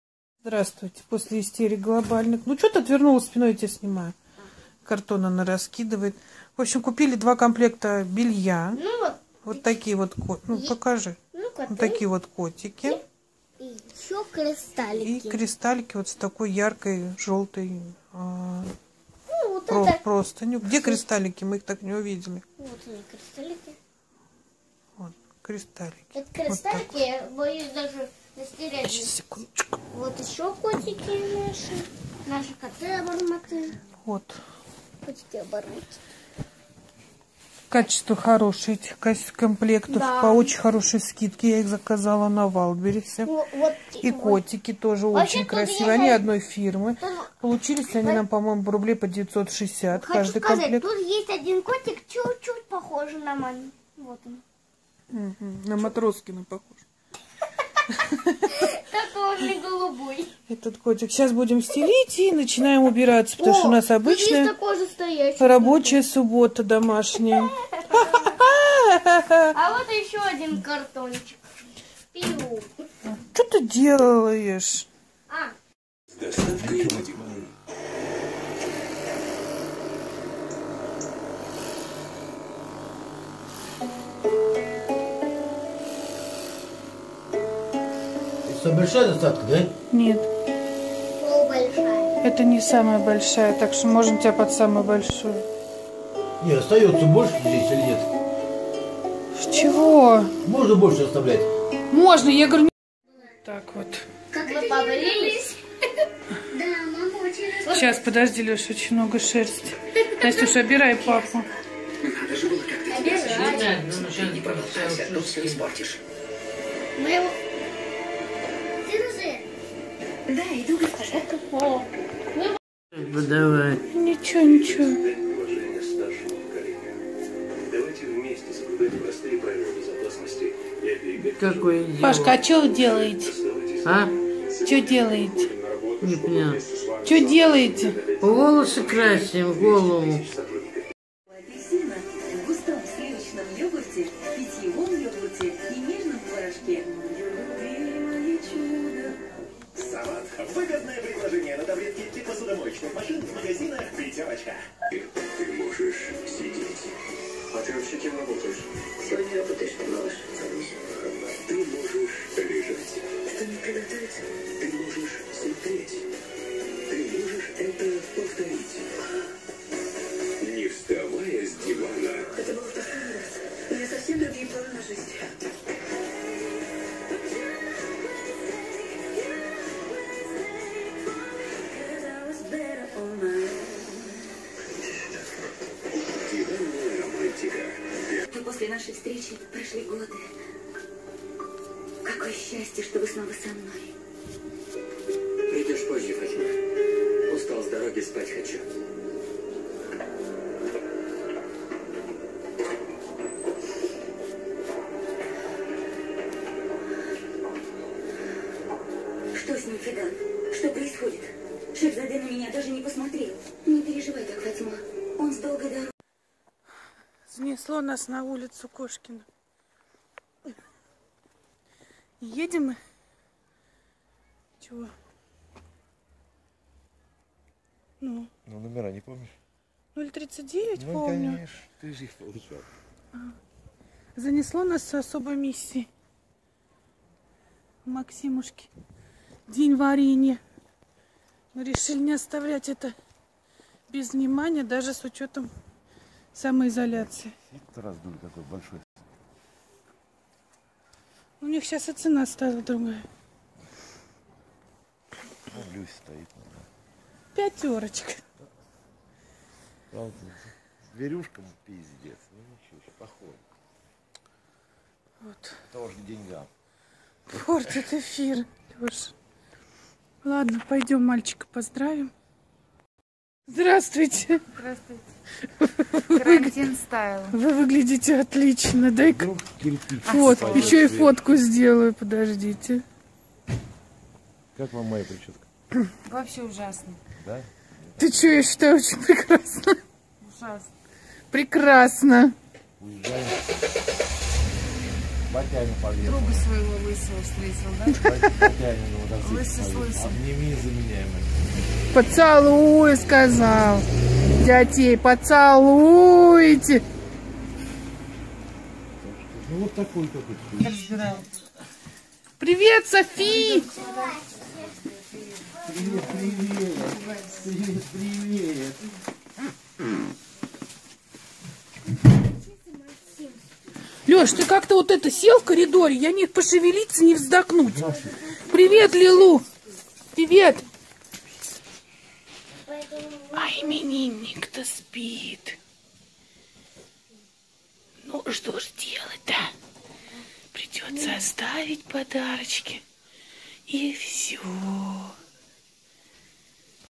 САЛОН КРАСОТЫ НА ДОМУ!
Как мы красоту дома наводили. Лешку под машинку. Маму в шапочку. Купили новое постельное белье.